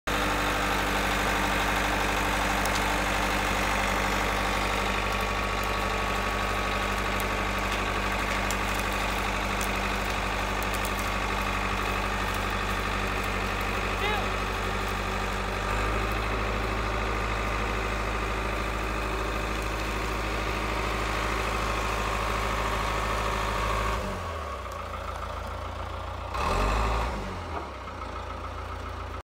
Caterpillar 931B
Pulling out some firewood with the old Cat 931B.

Heavy equipment, excavator, dozer, track loader.